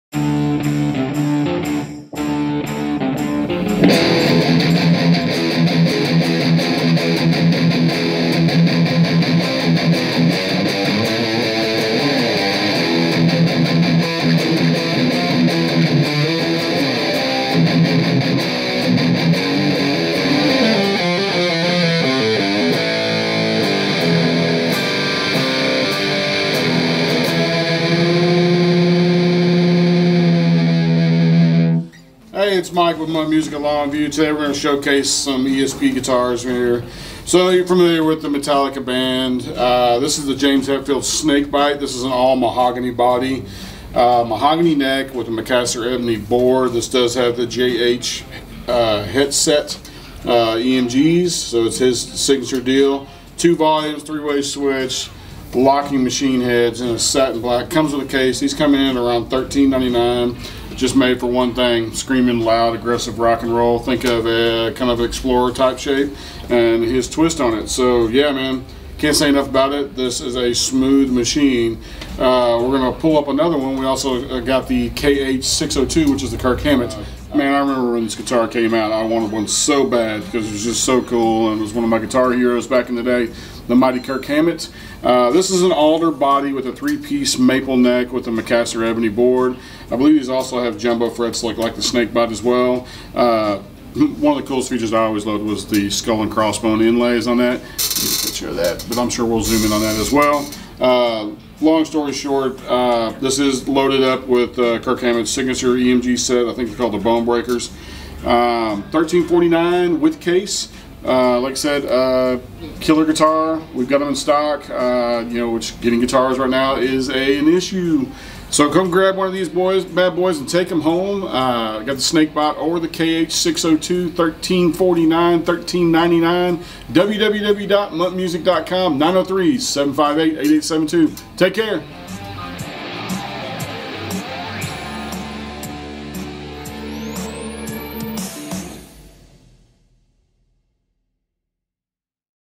ESP LTD Electric Guitars
LTD James Hetfield Snakebyte Electric Guitar & LTD KH-602 Kirk Hammett Signature Electric Guitar

James Hetfield Guitar Specs:

Number of Strings - 6
Left-/Right-handed - Right-handed
Body Type - Solidbody
Body Shape - Snakebyte
Body Material - Mahogany
Body Finish - Polyurethane Satin
Color - Black Satin
Neck Material - 3-piece Mahogany
Neck Shape - Thin U
Neck Joint - Set Neck
Radius - 13.7"
Fingerboard Material - Ebony
Fingerboard Inlay - Snakebyte with Dots
Number of Frets - 22, XJ
Nut Material - Molded Plastic
Bridge/Tailpiece - Tonepros locking TOM
Tuners - LTD locking
Neck Picku -  EMG JH-N Active Humbucker
Bridge Pickup - EMG JH-B Active Humbucker
Controls - 2 x volume
Switching - 3-way toggle pickup switch

Kirk Hammett Guitar Specs:

Left-/Right-handed - Right-handed
Body Type - Solidbody
Body Shape - Signature
Body Material - Alder
Body Finish - Gloss Polyurethane
Color - Black
Neck Material - 3-piece Maple
Neck Shape - Extra Thin U
Neck Joint - Neck-through
Radius - 13.7"
Fingerboard Material - Pau Ferro
Fingerboard Inlay - Skull and Crossbones
Number of Frets - 24, Extra Jumbo
Nut Material - Locking
Bridge/Tailpiece - Floyd Rose 1000 Locking Tremolo
Tuners - LTD
Neck Pickup - EMG Bone Breaker Active Humbucker
Bridge Pickup - EMG Bone Breaker Active Humbucker
Controls - 2 x volume, 1 x master tone
Switching - 3-way blade pickup switch

Get these limited guitars today!